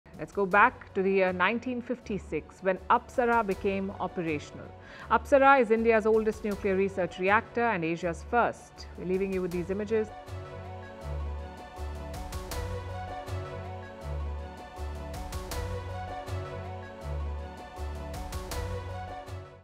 August 4, 1956: Asia's First Nuclear Research Reactor Becomes Operational In India |Firstpost Rewind
Nuclear Research Reactor APSARA was commissioned by Bhabha Atomic Research Centre in India's Maharashtra. APSARA is India's oldest and Asia's first Nuclear Research Reactor.

India | Nuclear Reactor | Apsara | Firstpost | World News | Latest News | Global News | International News | Trending News